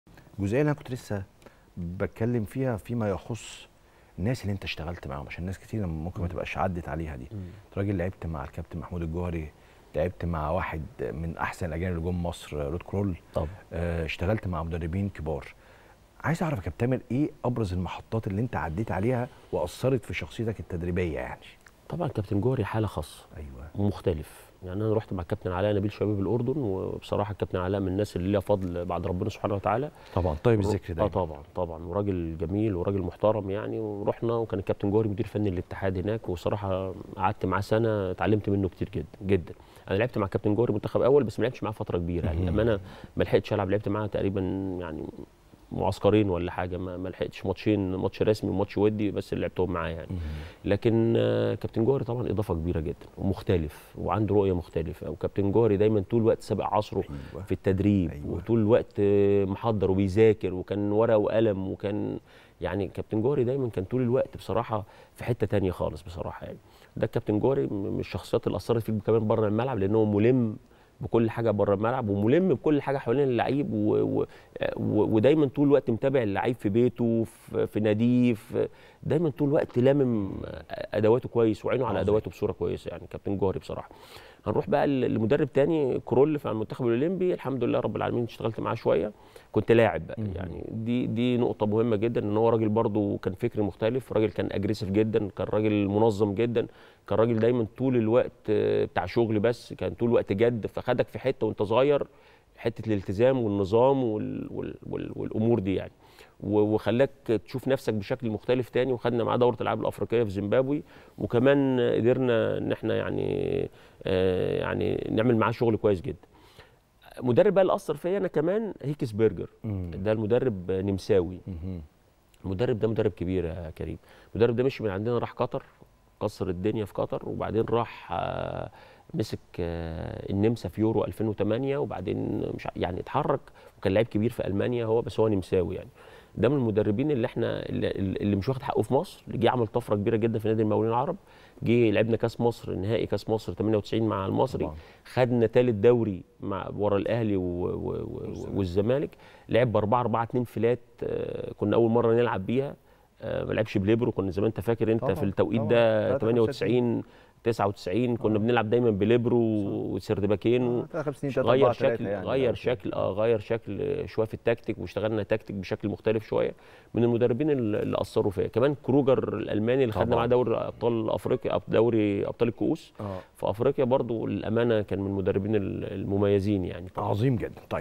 تامر مصطفى يحكي عن كواليس تعامله مع الجوهري: كان سابق عصره وهذا المدرب عمل طفرة في مصر ومش واخد حقه
رقم 10 | تامر مصطفى يحكي عن كواليس تعامله مع الجوهري: كان سابق عصره.. وهذا المدرب عمل طفرة في مصر ومش واخد حقه 👇🏻💥